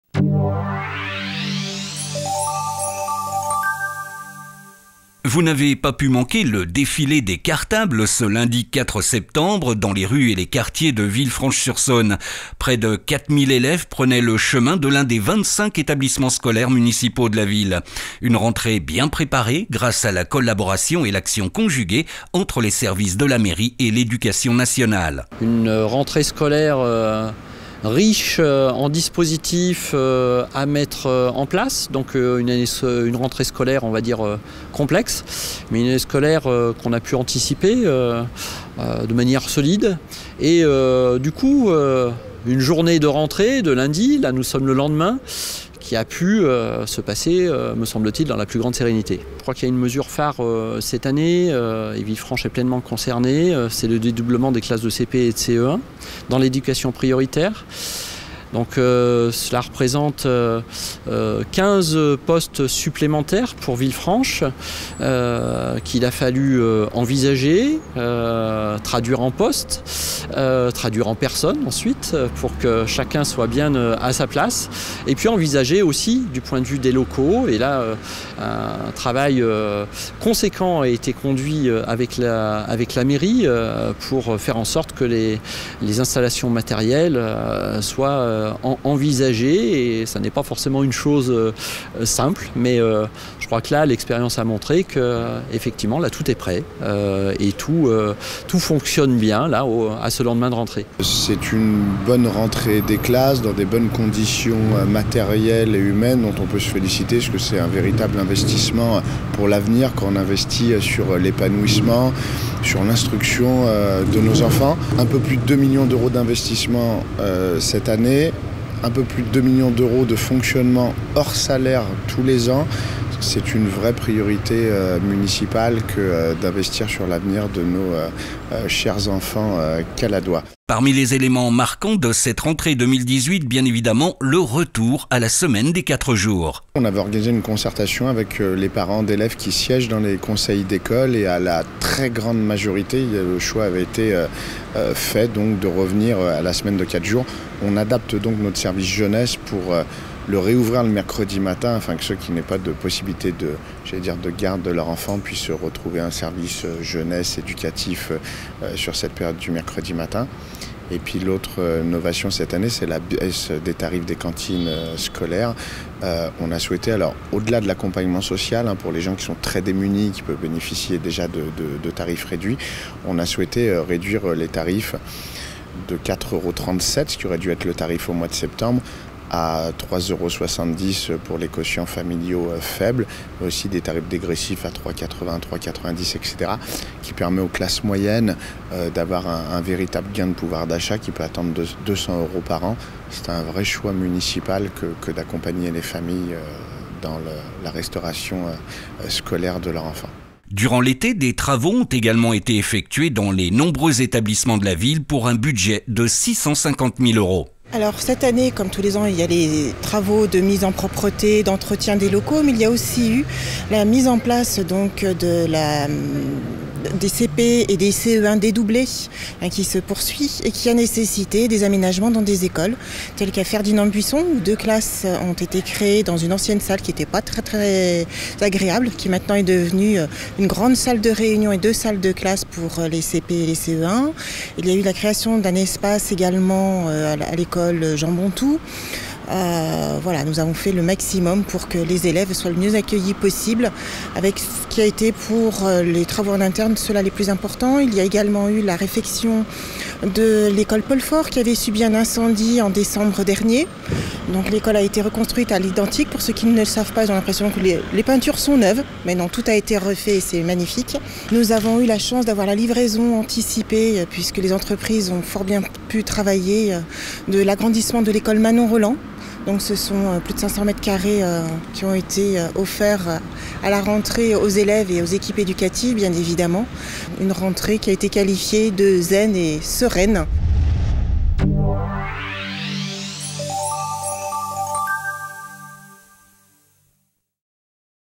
Rentrée Scolaire 2018 à Villefranche-sur-Saône
Rentrée scolaire à Villefranche-sur-Saône.
Semaine de 4 jours, dédoublements de classes, cantine, travaux... quels sont les nouveautés et les moyens qui ont été mis en oeuvre par la Ville de Villefranche et l'Éducation Nationale ?